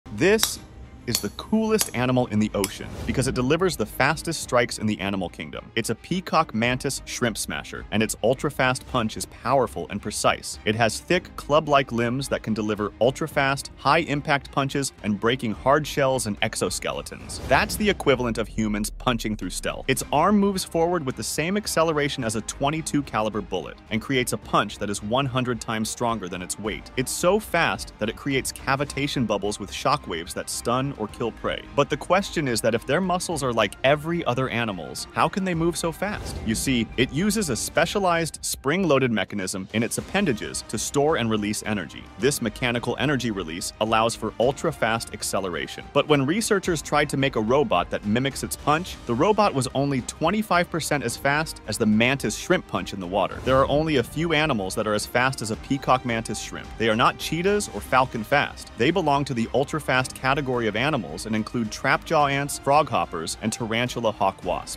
Mantis Shrimp Smasher: Punching Harder Than Steel!
Meet the Peacock Mantis Shrimp, the ocean’s ultimate predator with the fastest punch in the animal kingdom! This tiny powerhouse strikes with the force of a bullet and enough power to crack hard shells and exoskeletons. But what makes this punch so fast? It’s all thanks to a spring-loaded mechanism in its limbs that releases energy for ultra-fast acceleration. Discover how this shrimp’s punch can even outperform robots designed to mimic it!